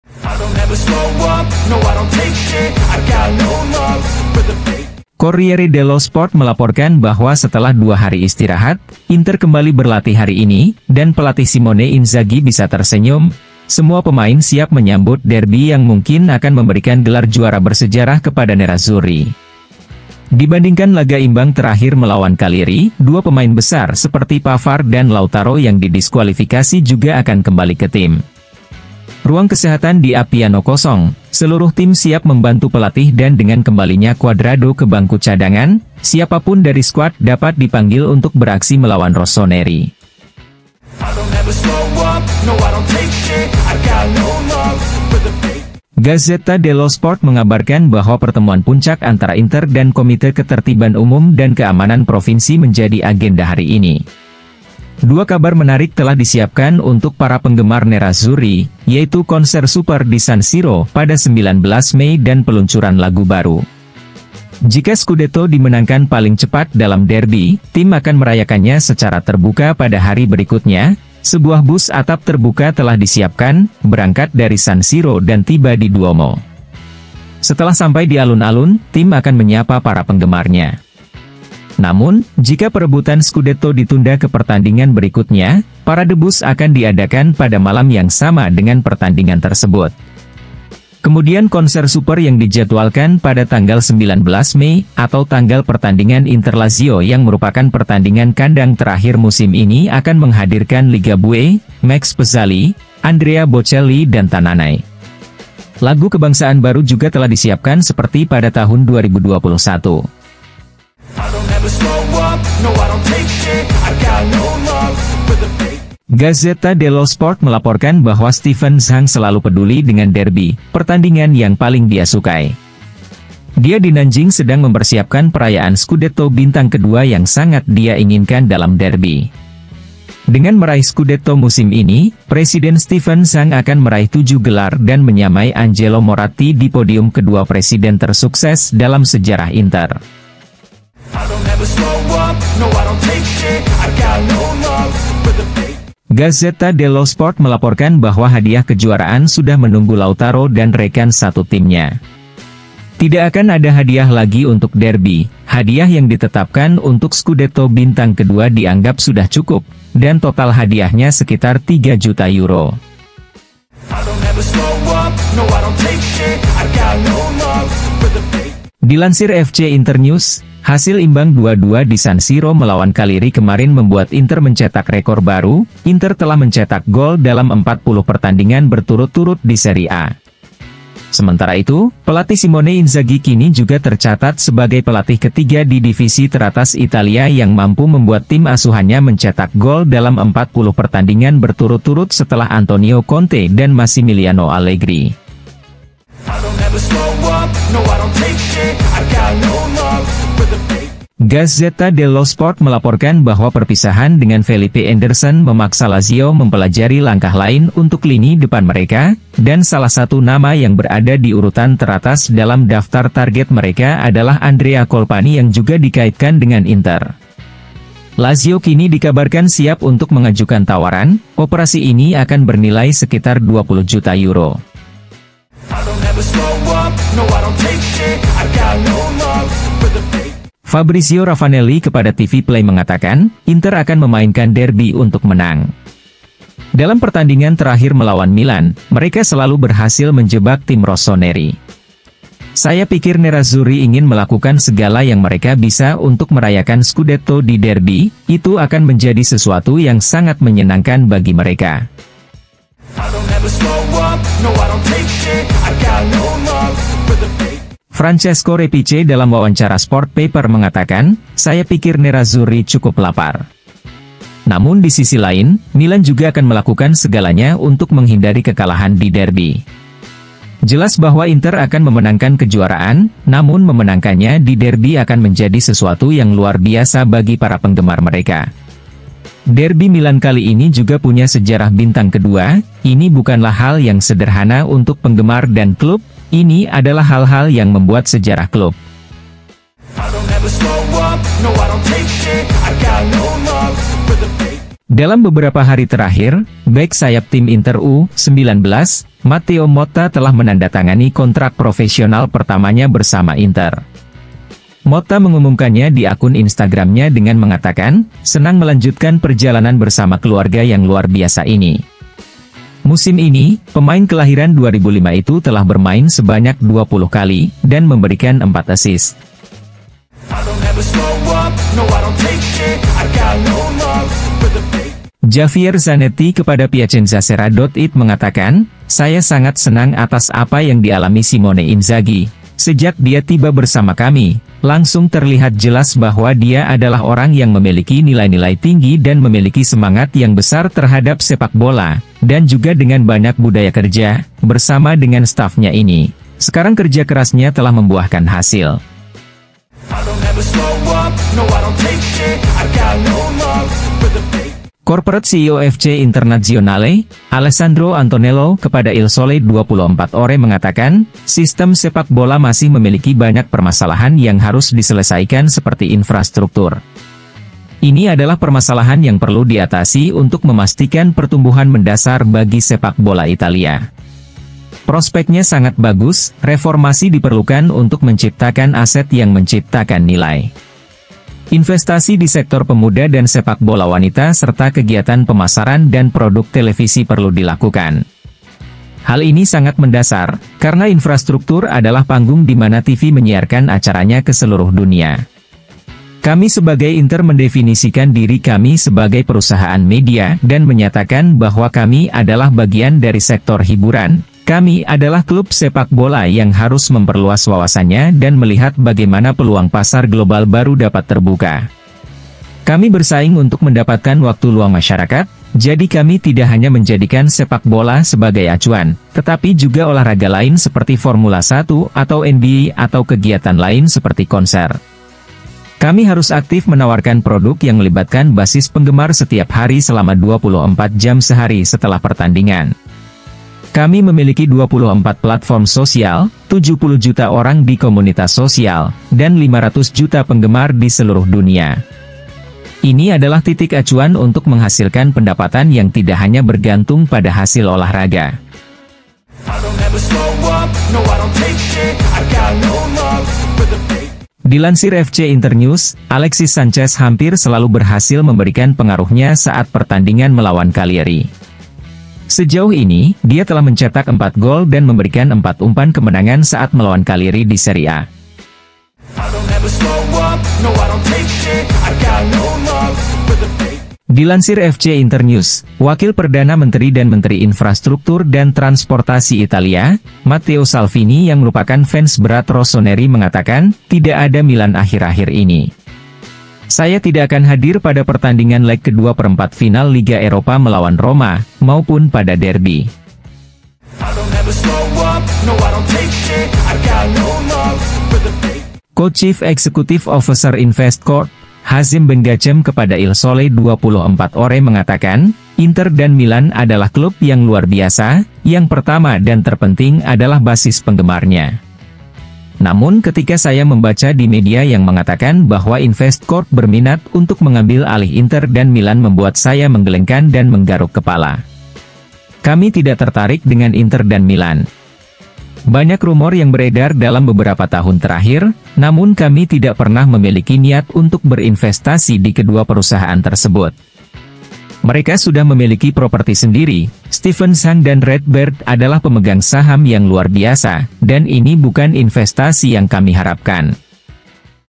KONSER SUPER DAN LAGU BARU | BONUS SCUDETTO | STEVEN ZHANG | SIMONE INZAGHI | ZANETTI | ANTONELLO
- SEMUA PEMAIN SIAP BANTU SIMONE INZAGHI
- KONSER SUPER DAN LAGU BARU
- STEVEN ZHANG PERSIAPKAN PERAYAAN SCUDETTO BINTANG KEDUA DI NANJING
- HADIAH SCUDETTO SENILAI 3 JUTA EURO
- REKOR BARU INTER DAN SIMONE INZAGHI
- LAZIO SIAPKAN TAWARAN UNTUK ANDREA COLPANI
- RAVANELLI: INTER MENGINGINKAN SCUDETTO DI DERBY
- REPICE: MILAN AKAN LAKUKAN SEGALANYA UNTUK MENGHINDARI KEKALAHAN DI DERBY
- KONTRAK PROFESIONAL PERTAMA MATTEO MOTTA
- ZANETTI: SANGAT SENANG DENGAN APA YANG DIALAMI SIMONE INZAGHI
- ANTONELLO: INFRASTRUKTUR ADALAH PERMASALAHAN YANG PERLU DIATASI
- CAGLIARI MENJADI TIM YANG PALING DISUKAI ALEXIS SANCHEZ
- SALVINI: TIDAK ADA MILAN AKHIR-AKHIR INI
- BEN-GACEM: STEVEN ZHANG ADALAH PEMEGANG SAHAM YANG LUAR BIASA